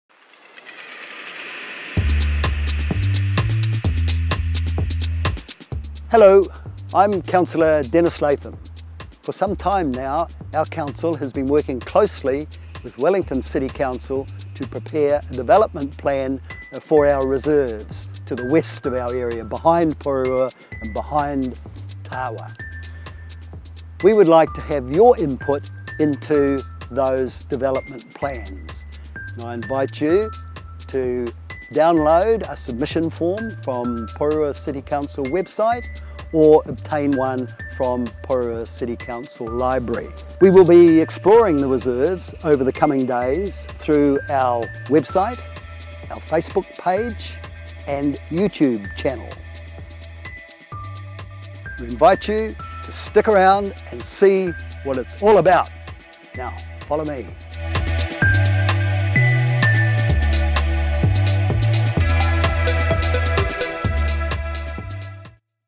Porirua's outdoor recreation park | Consultation video | Denys Latham introduction
Councillor Denys Latham introduces Porirua's proposed outdoor recreation park consultation process and the video series that follows.

The consultation period runs from 21 September until 21 October 2015.

Find out more about Porirua and the Porirua City Council by visiting us on Facebook, Twitter and online through our website.